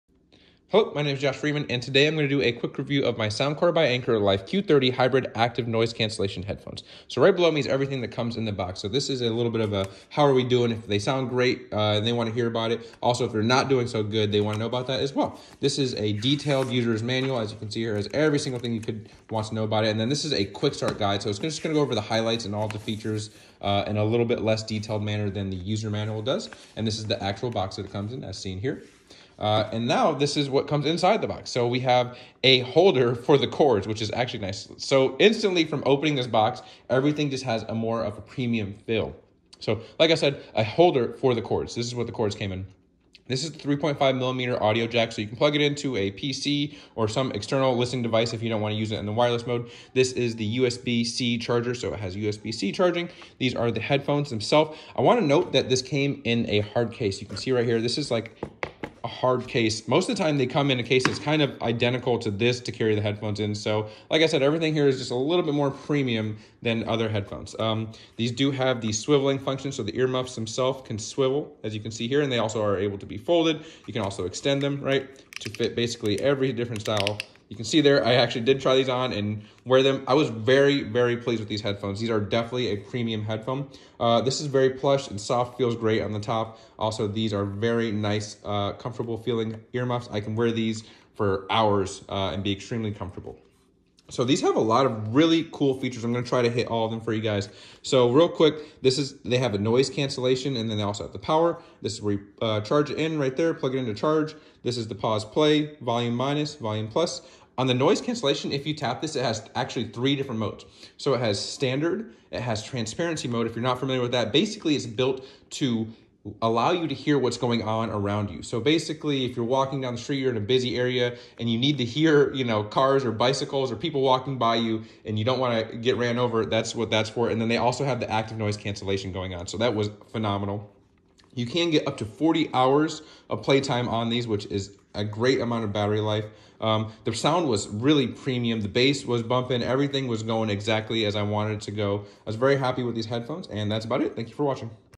Anker Soundscore Q30 Quick Review
Anker Soundcore Q30 headphones are the level above the Q20's. There is a notable difference in quality instantly from the packaging to the materials and sound quality.